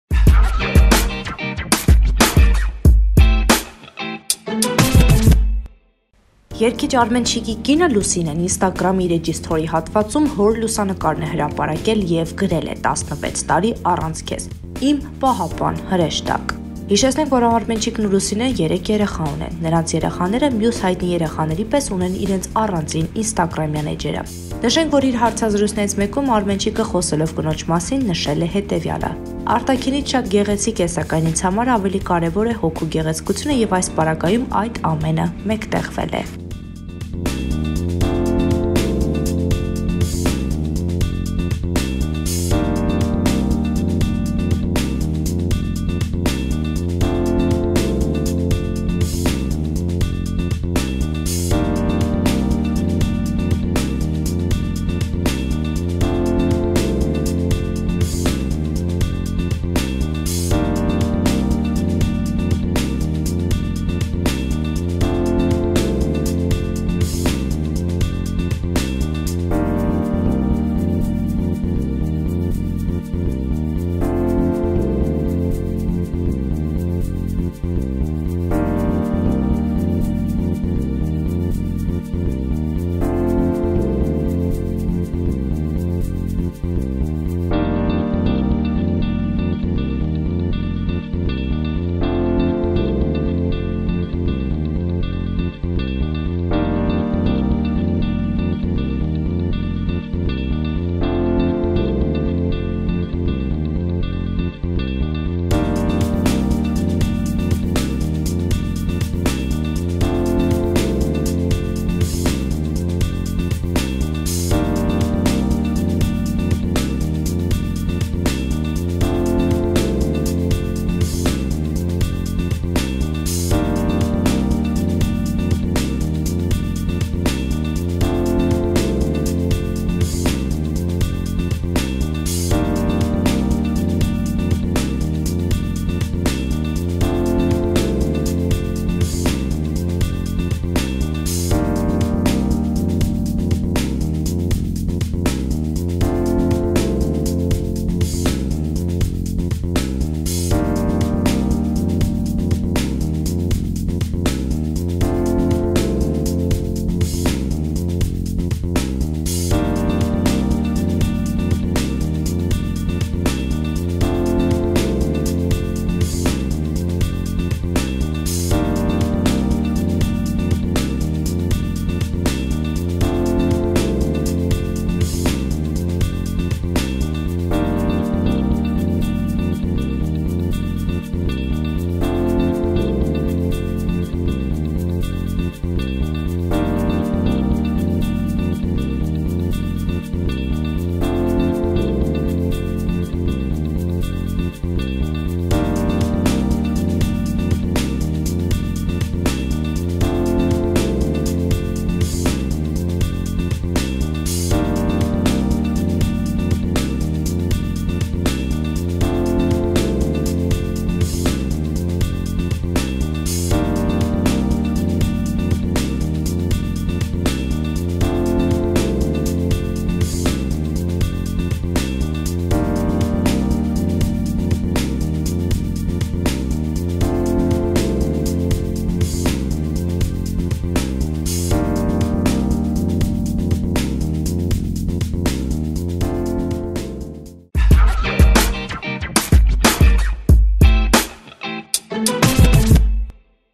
16 տարի առանց քեզ
Արմենչիկն ու Լուսինեն 3 երեխա ունի: Նրանց երեխաները մյուս հայտնի երեխաների պես ունեն ինստագրամյան էջեր:Նշենք, որ իր հարցազրույցներից մեկում Արմենչիկը, խոսելով կնոջ մասին, նշել էր.

«Արտաքինից շատ գեղեցիկ է, սակայն ինձ համար ավելի կարեւոր է հոգու գեղեցկությունը, եւ այս պարագայում այդ ամենը մեկտեղվել է»:

Մանրամասն՝ տեսանյութում